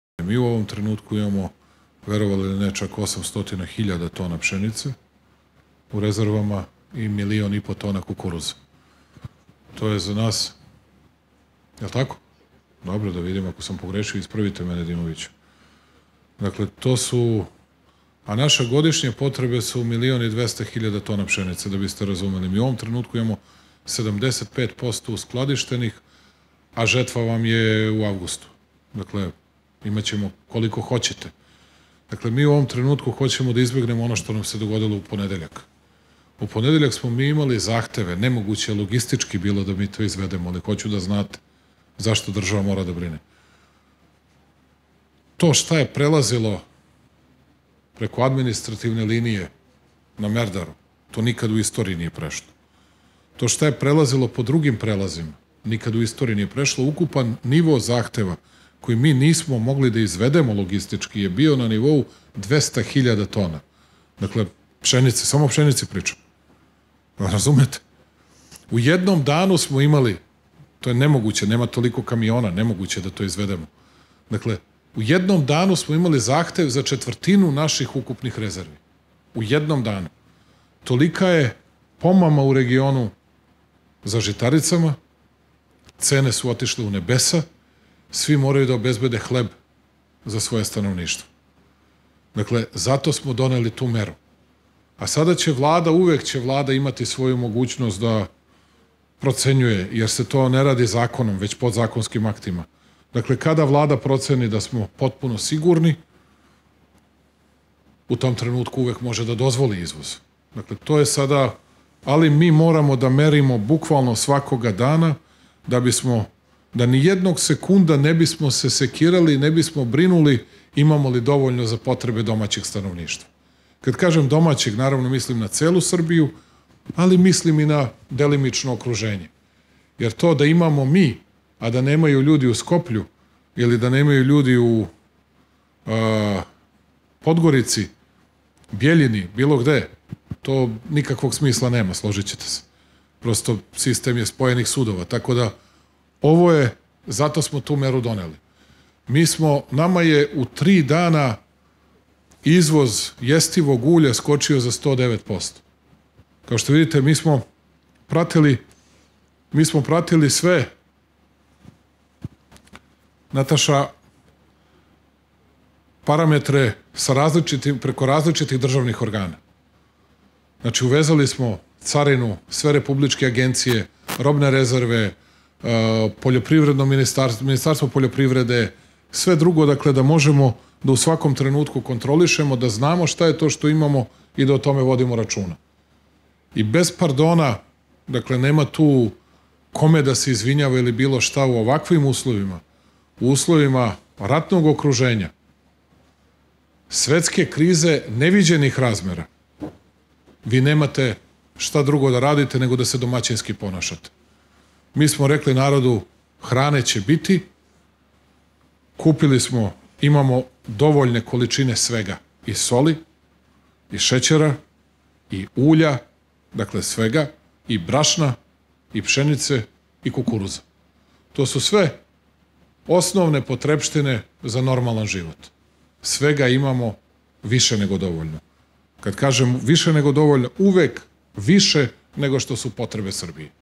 O zalihama osnovnih životnih naminnica
U rezervi imamo dovoljne količine osnovnih životnih namirnica“  naglasio je , izmeđuostalog  predsednik Srbije Aleksandar Vučić prilikom nedavne posete Šidu. On je ujedno obrazložio predlog mere zabrane izvoza žitarica